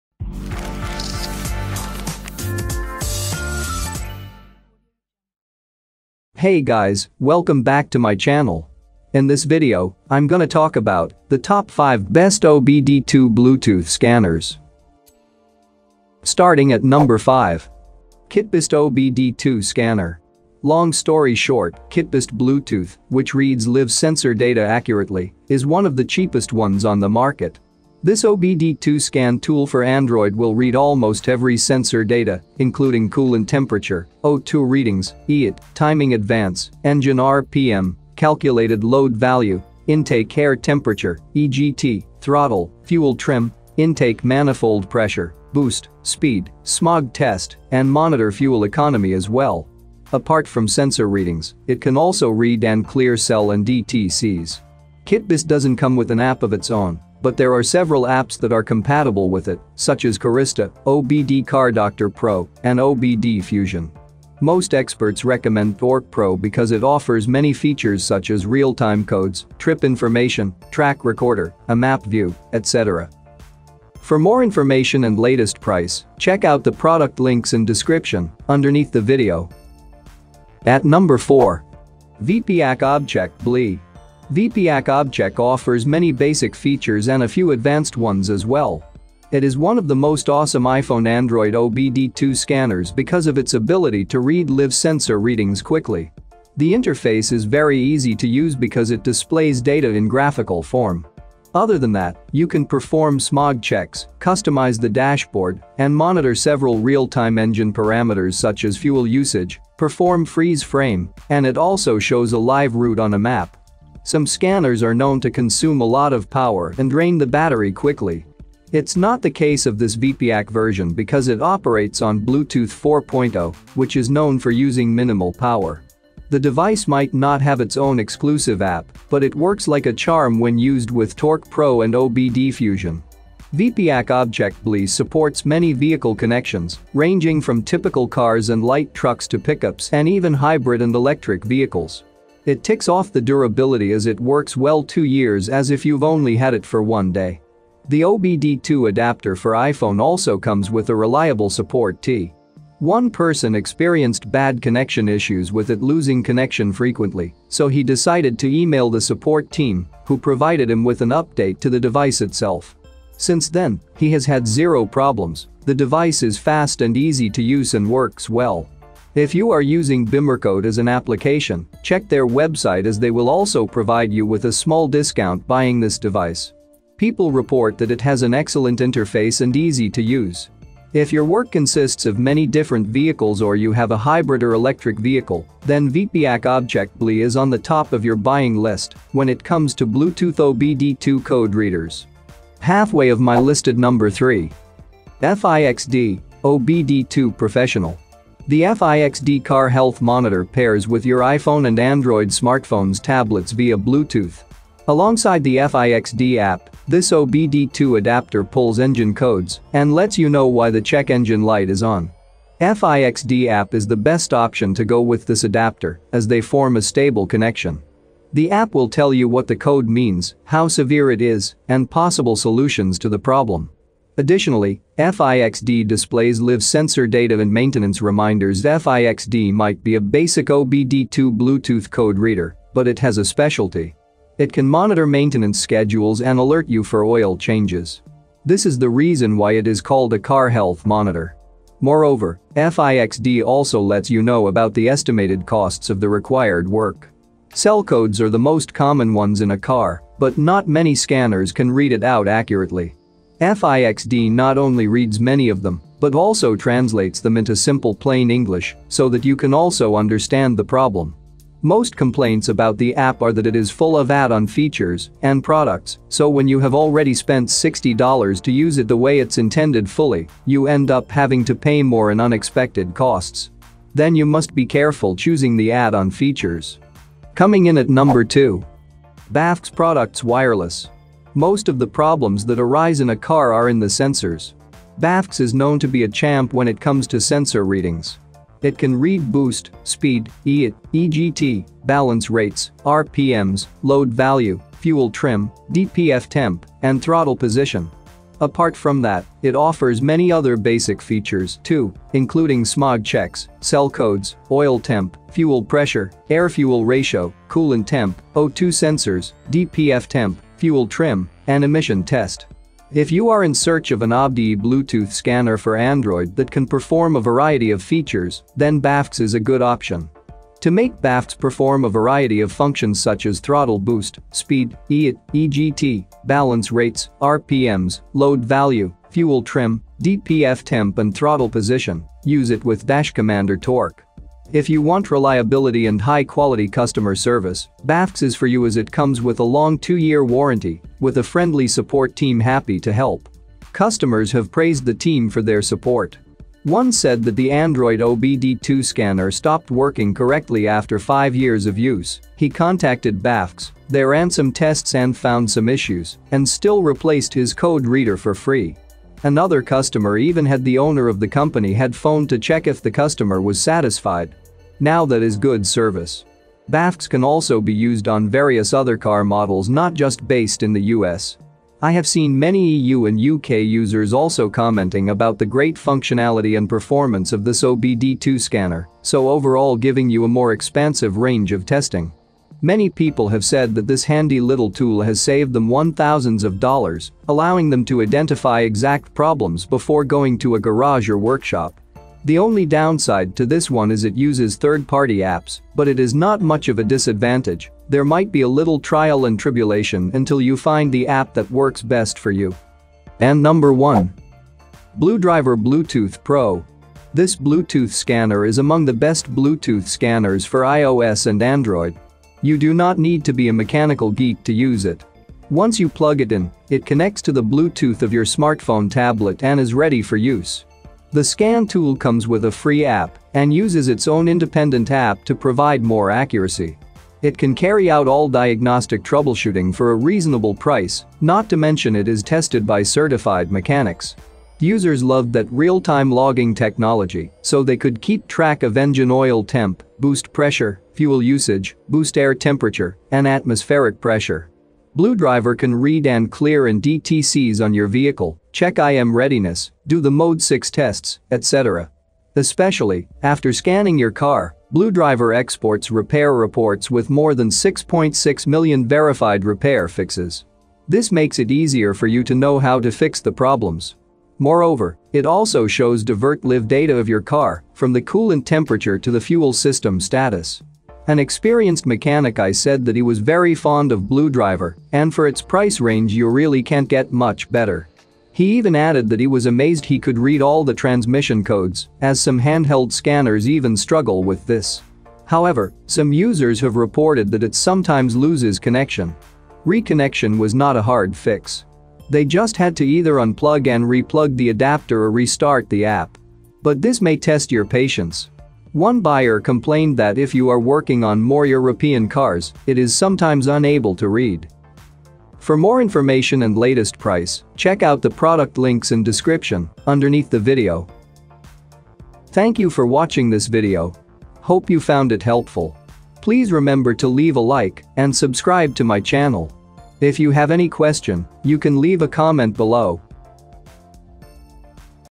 Top 5 Best Obd2 Bluetooth Scanners Reviews 2023
Here is the Updated Prices of the Best Multimeter in 2023!

Things that we mentioned in this video:
1. Fluke 115 Compact True RMS - B01EWFB9FI
2. INNOVA 3320 - B000EVYGZA
3. INNOVA 3340 Automotive Digital - B000KIMHRQ
4. Fluke 116 HVAC Multimeter - B000NI69YA
5. Digital Multimeter Auto Ranging 600V - B018EXZO8M